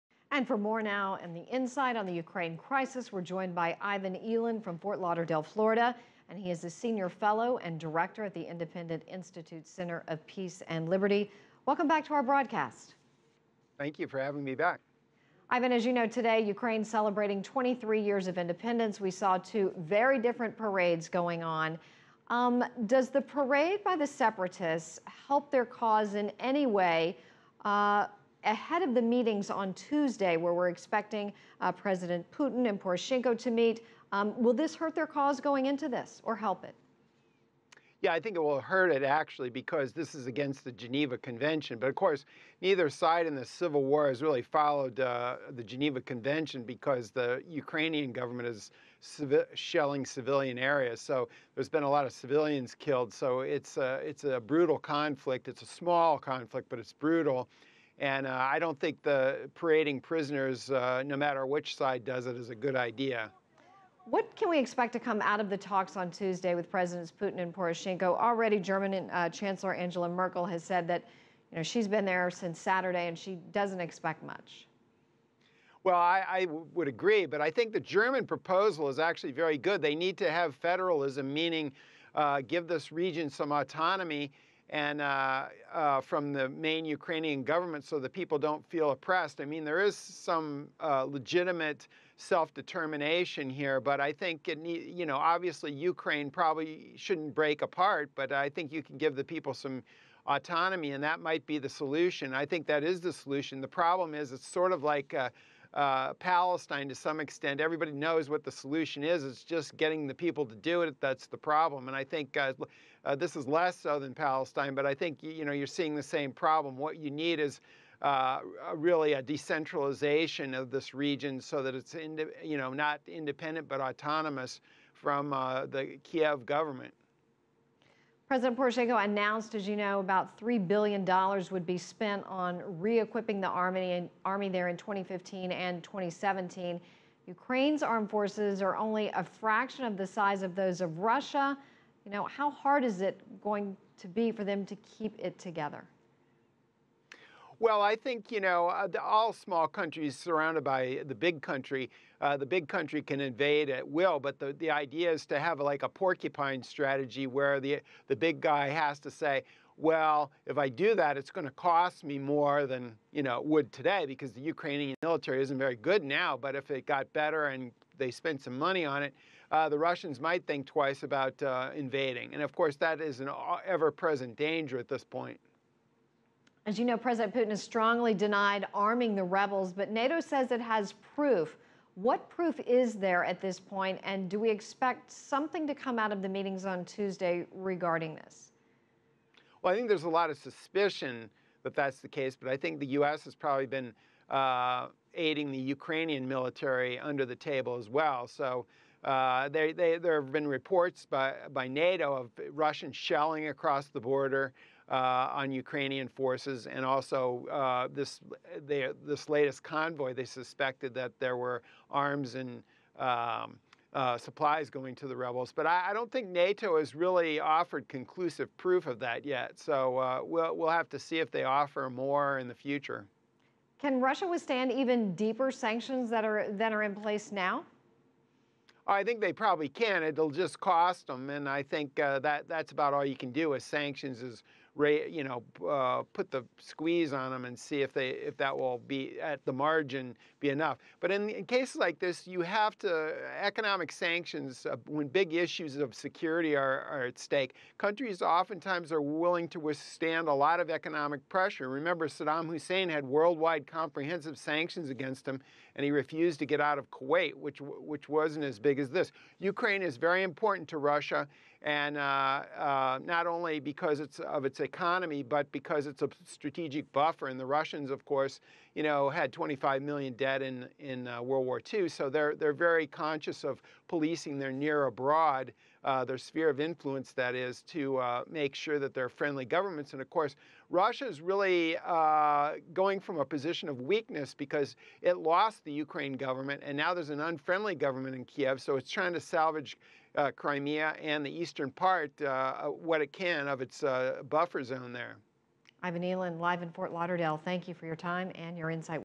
What's next for Russia-Ukraine relations?
For more insight on the Ukraine crisis, Ivan Eland joined CCTV America. He is the director at the Independent Institute's Center of Peace and Liberty.